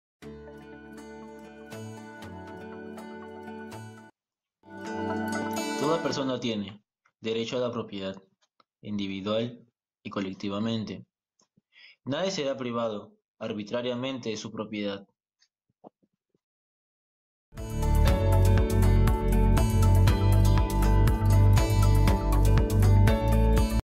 Sebastián Andrés Marthans Delzo, Peru, reading article 17 of the UDHR
To raise awareness for basic human rights, the United Nations is asking people around the world to record themselves reading articles of the Universal Declaration of Human Rights in their language.